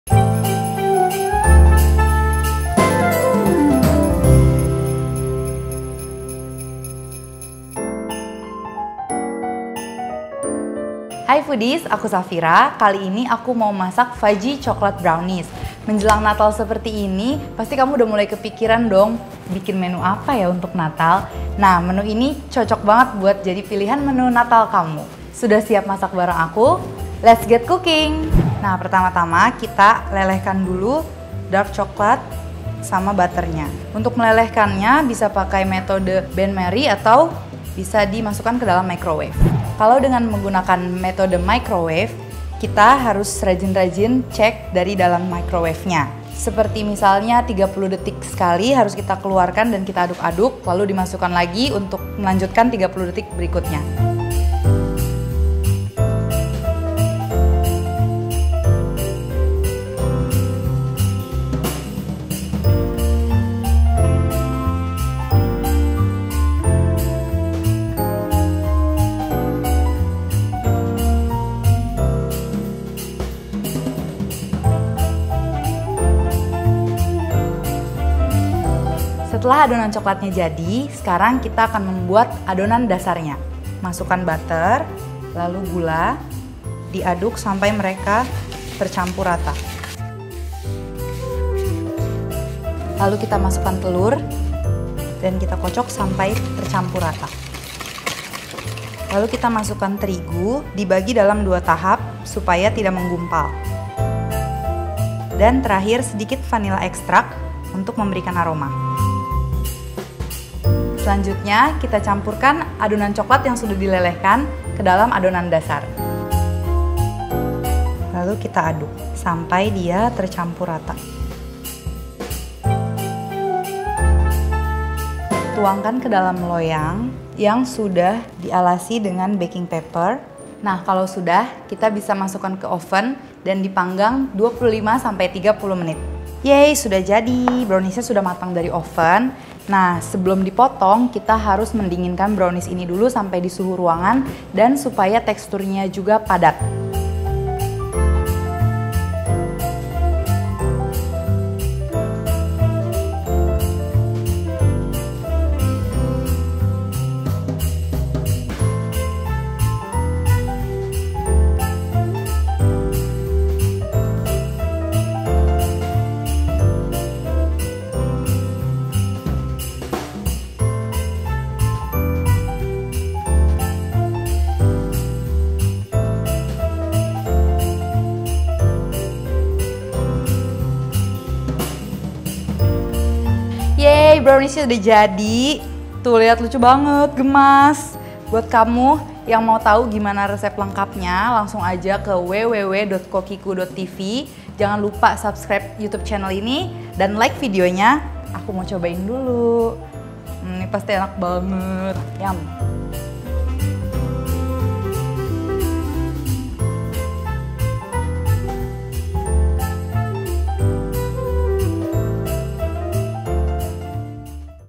Resep Fudgy Chocolate Brownies | SAVIRA PRADIATI
Hai Foodies, Savira mau share resep Fudgy Chocolate Brownies yang yummy banget nih. Beda dengan brownies biasa loh. Mau tahu? Let's get cooking!